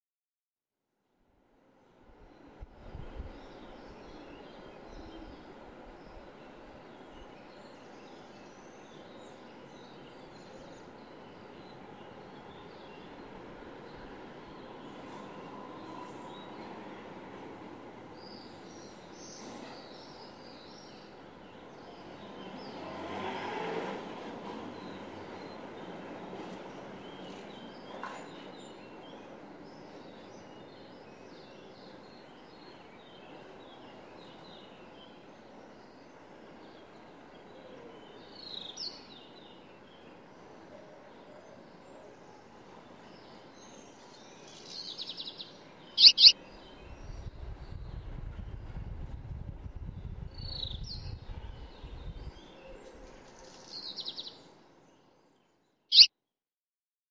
Yellow Wagtail
Yellow Wagtail preening in the S'Albufera region of Majorca Spain during May